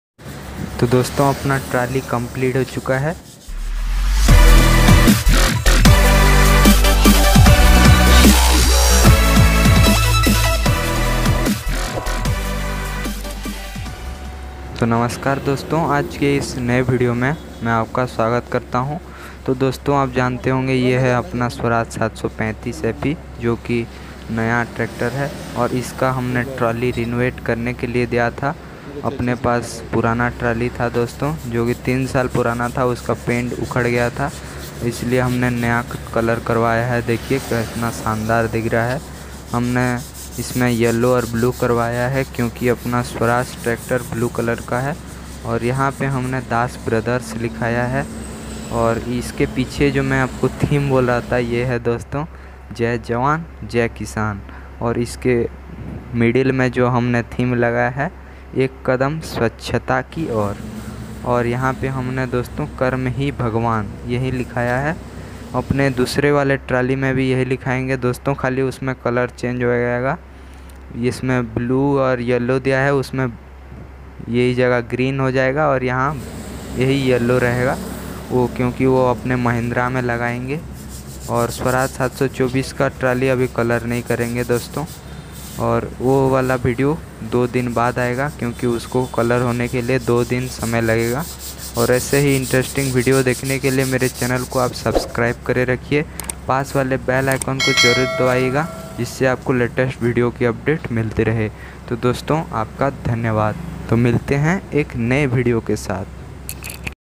My new tractor trolley || tractor trolley painting | How to paint tractors trolley#Sailabsekhardash
About vedio : I am sailab sekhar dash in today vedio I show you my new tractor trolley and it's review

1. my new tractor trolley
2. my new tractor trolley painting
3. tractor trolley painting
4. tractor trolley modification
5. Tractor trolley colouring and painting

previous vedio link